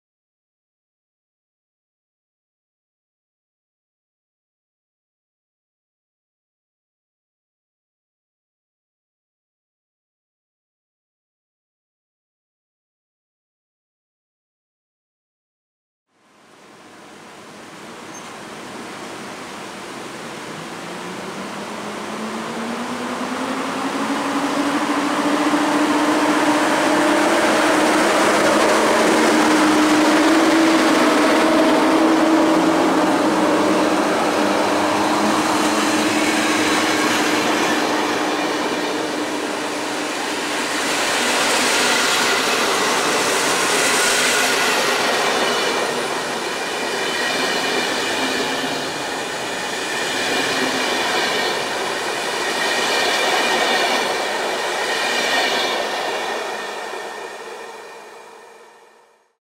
EU07-033 PKP Cargo z IC 56102/3 Mamry do Wrocławia Głównego.
EU07-033 PKP Cargo wynajęta dla PKP Intercity odjeżdża z peronów stacji Żmigród z pociągiem IC 56102/3 Mamry relacji Białystok - Wrocław Główny.

Województwo: Dolnośląskie
Miejscowość: Żmigród
Data: 25 Lipiec 2021r.
Godzina: 19:20
Przewoźnik: PKP Cargo, PKP Intercity
D29: 271 (Wrocław - Poznań)
Montaż: Camtasia Studio